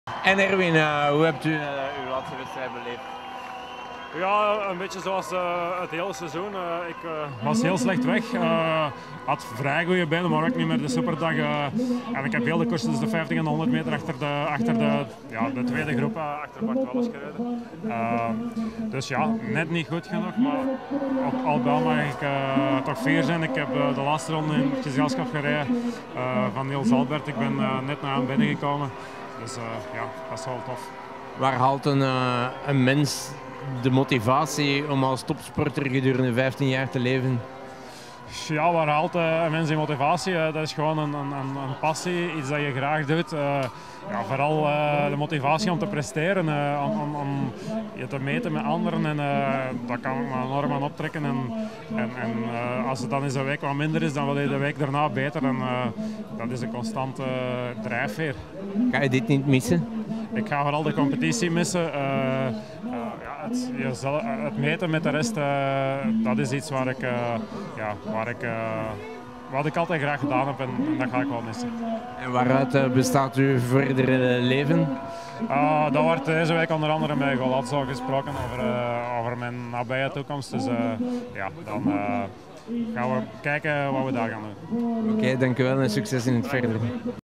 Erwin Vervecken 2 (Internationale Sluitingswedstrijd Cyclocross 2010)
Interview van Erwin Vervecken tijdens de Internationale Sluitingswedstrijd Cyclocross 2010